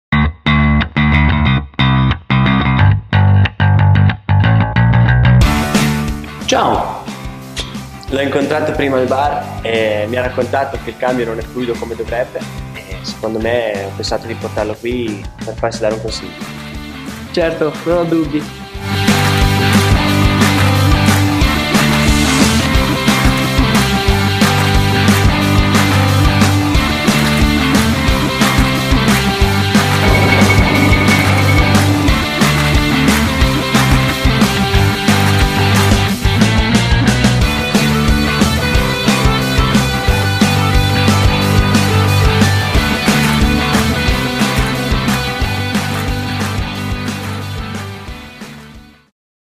DELLORTO - Eni i-Ride ROTRA con Pecco Bagnaia e Dennis Foggia
I ragazzi dello Sky Racing Team VR46 sono così a proprio agio nell'officina Dellorto che ormai dispensano consigli sui lubrificanti Eni in piena autonomia.